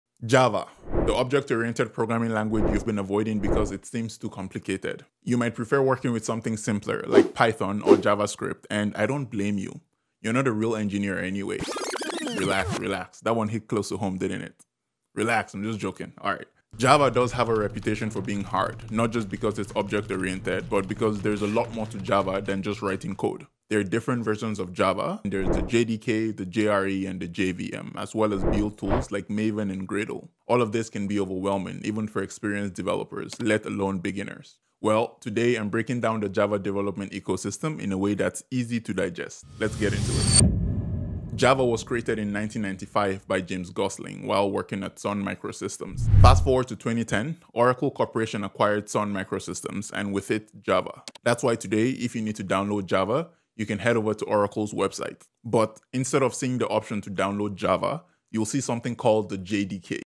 Java! The programming language for Real Software Engineers...
Java is one of those programming languages that seems complicated at first, but once you understand how the pieces fit together, it all starts to make sense.